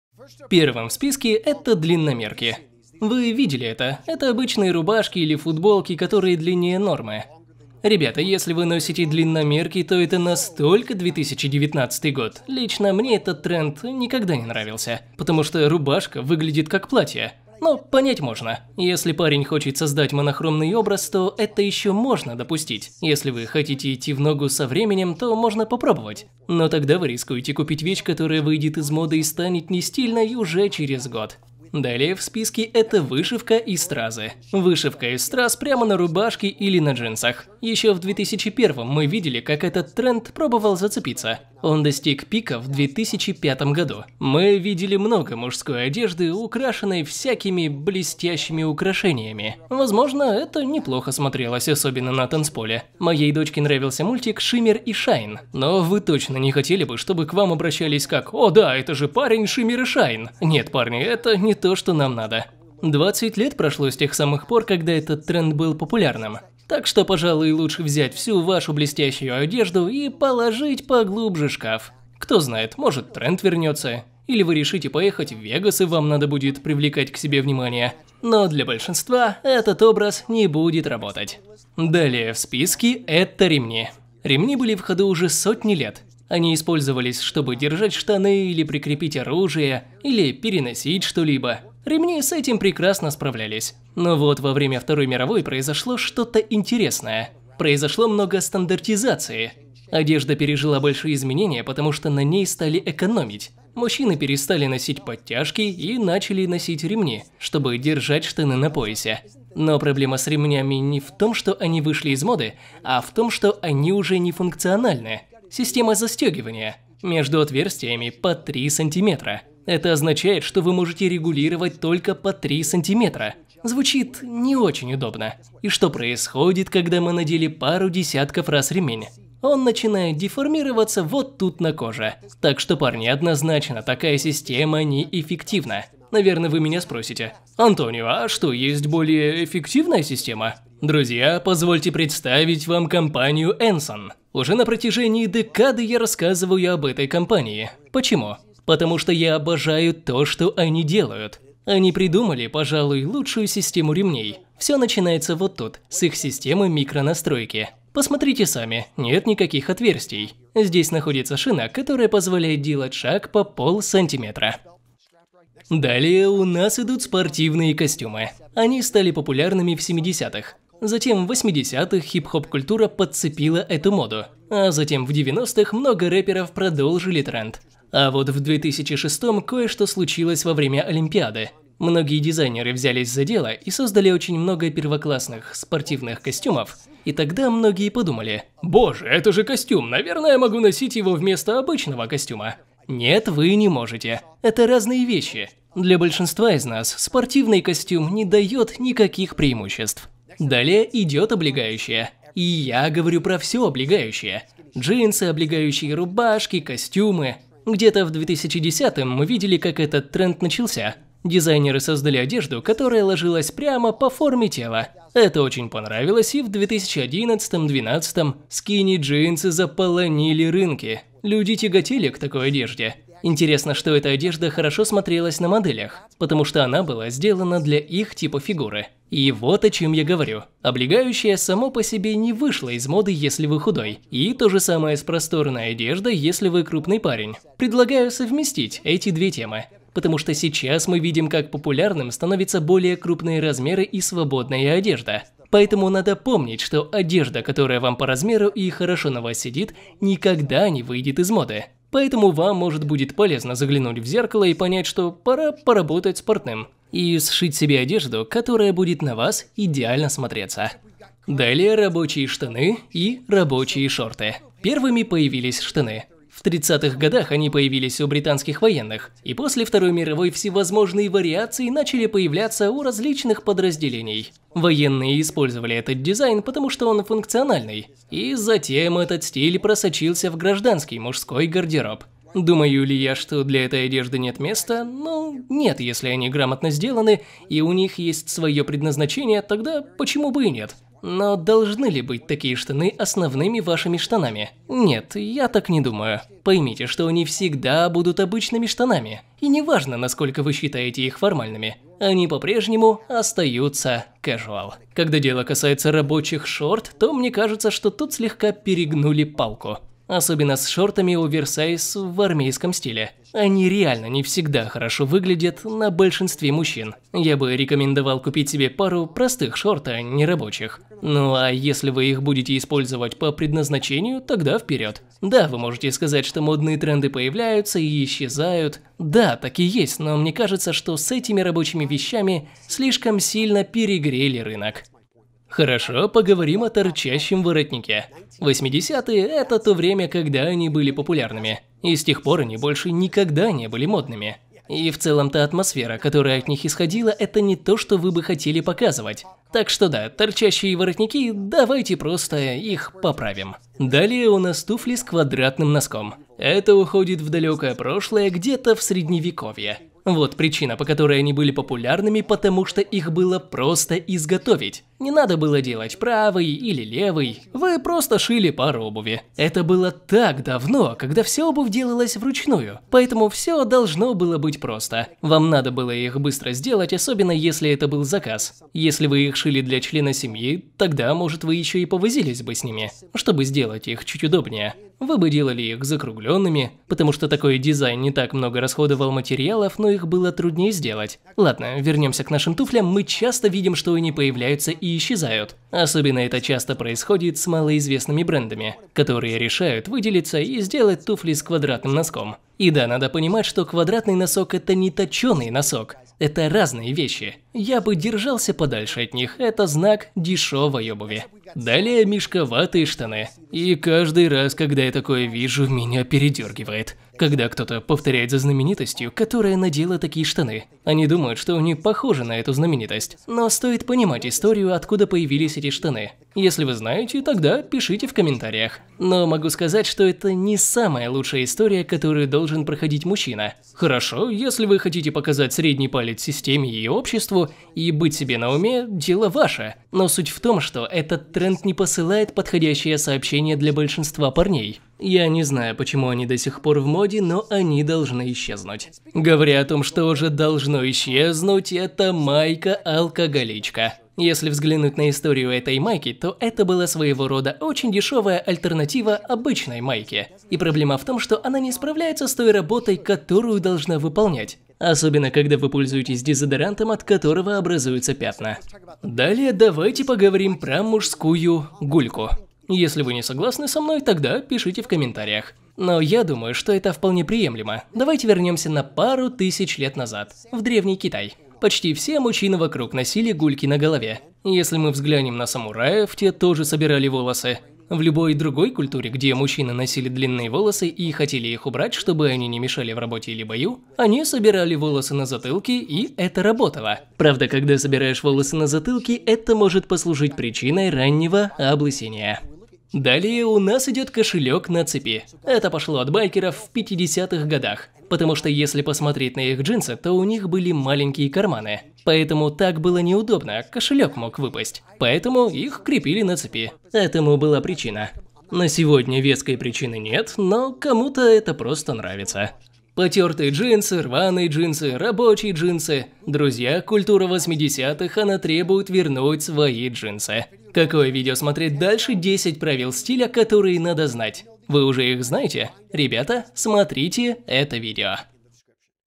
13 Устаревших Вещей в Мужском Гардеробе | Их НАДО Заменить
Какие вещи модные? Существует ли модная мужская одежда? Да, но нужна ли она вам? Тренды всегда меняются и устаревшие вещи становятся ненужными или преобразуются.

13 Устаревших Вещей в Мужском Гардеробе | Их НАДО Заменить

В большинстве своем вещи в мужском гардеробе появляются по необходимости. Конечно, некоторые из нас гоняются за трендами, т.к. смотрят разные ролики фэшн-блоггеров, которые говорят, как одеваться мужчине.

Купить модные мужской костюм или рубашку, которые потом будут висеть в гардеробе, и вы не сможете это носить. Модные мужские вещи – это коварная штука.

Гардероб мужчины должен состоять из разнообразных, взаимозаменяемых вещей, чтобы всегда быть стильным.